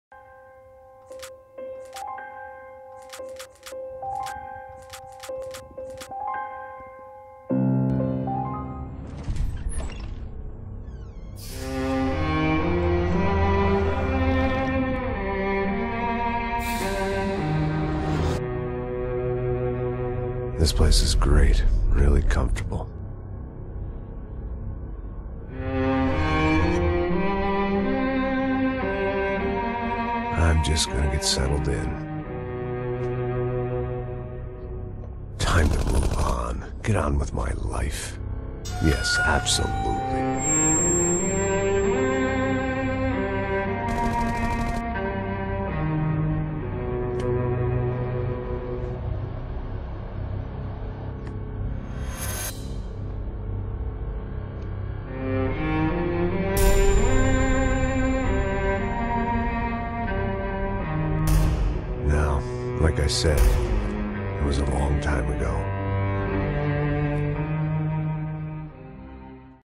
Nicolas Payne Intro - Hermanos Parro Trailer 2013
Próximamente "Los hermanos Parro 2013"
Fan Max Payne 3

Actores:
Nicolás Parro
Mikeparro